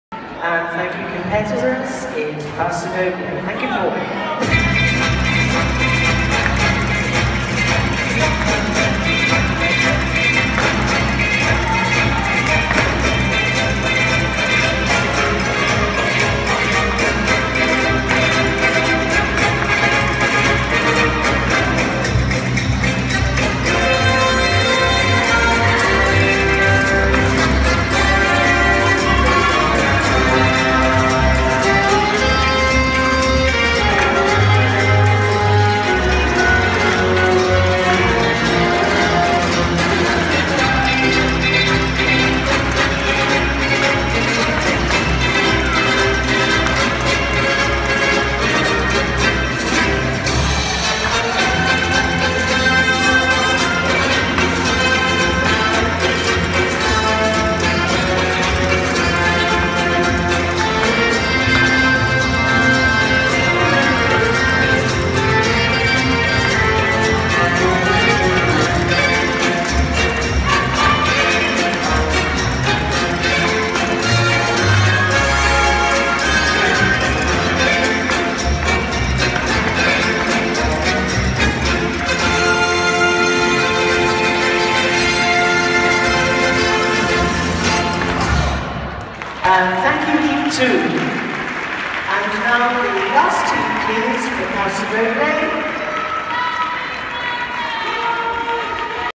Leonid Oleynik-Savina Anastasia pasodoble
European championship 2016 Latin junior under 16